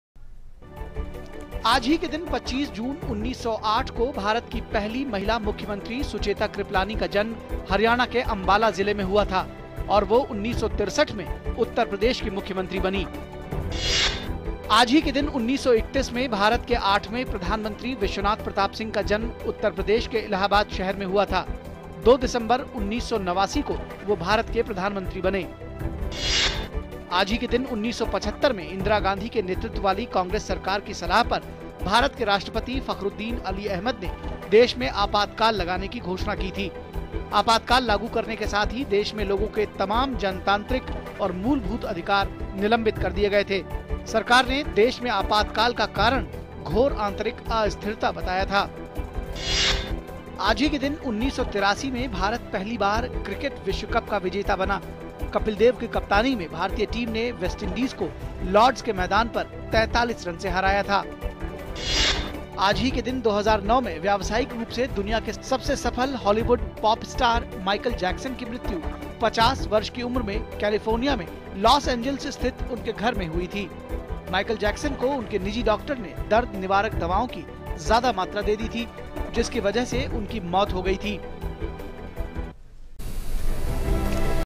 इतिहास के झरोखे से 25/06/2019
जानिए क्यों लगाया गया था 44 वर्ष पूर्व भारत में आपातकाल - देखें आज के इतिहास में।